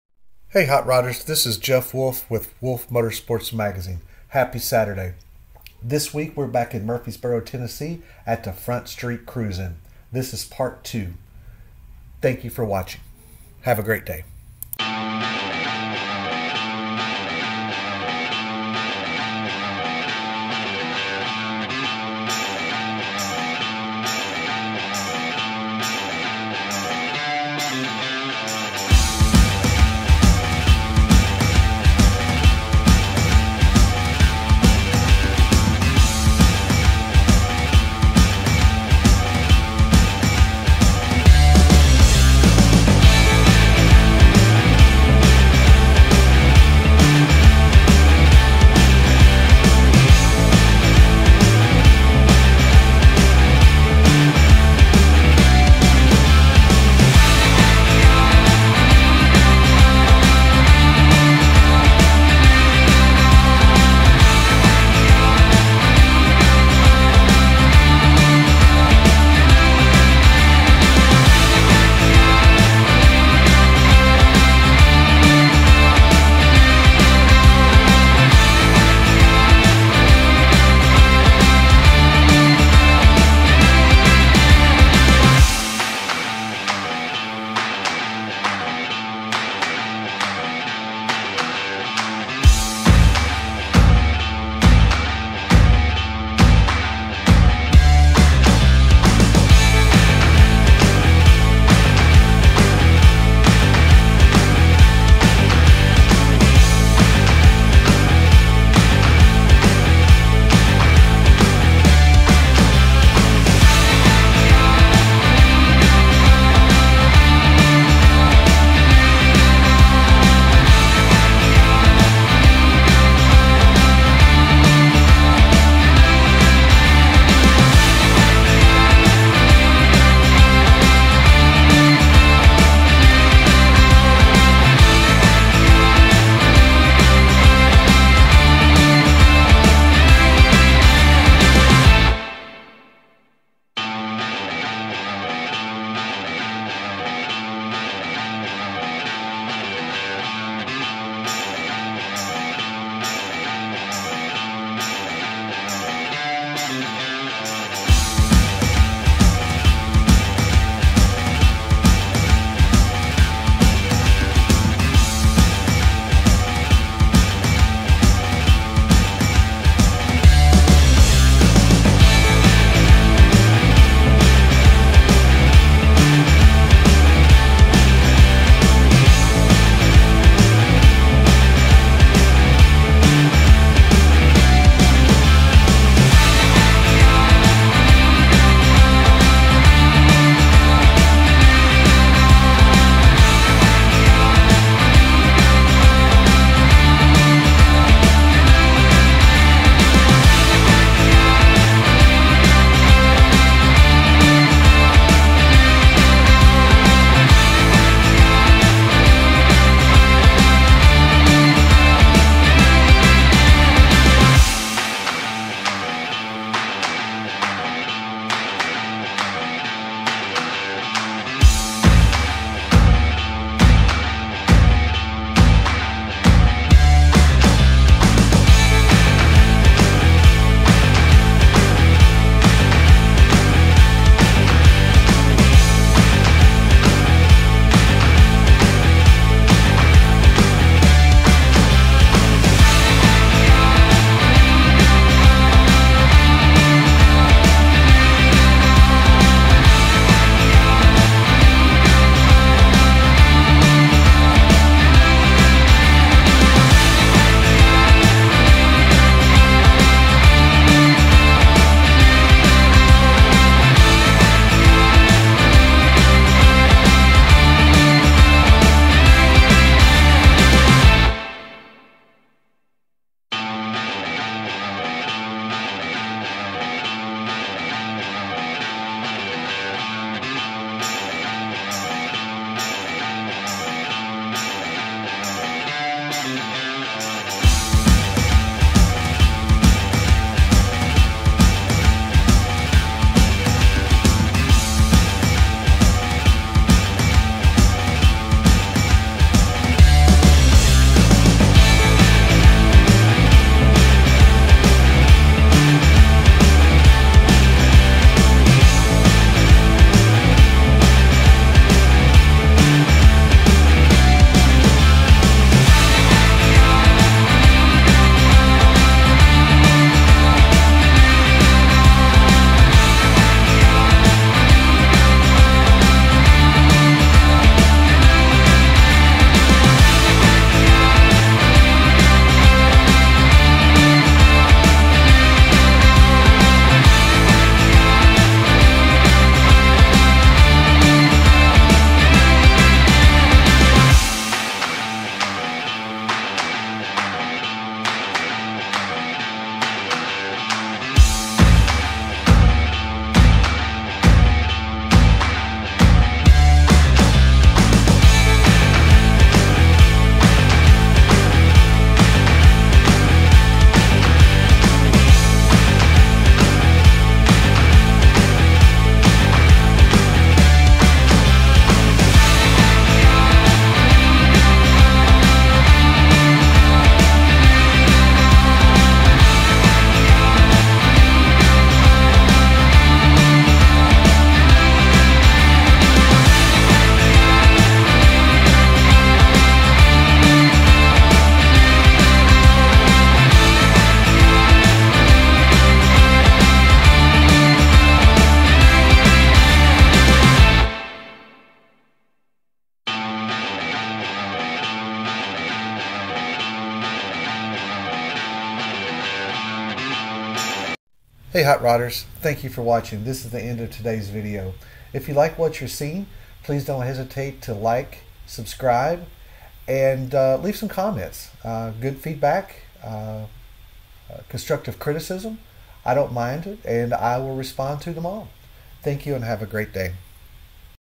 Front Street Cruise in Part 2 Murfreesboro, TN (classic cars, pick ups, & more)
This is Part 2 in a 4 part series from The Front Street Cruise-in in Murfreesboro, TN!!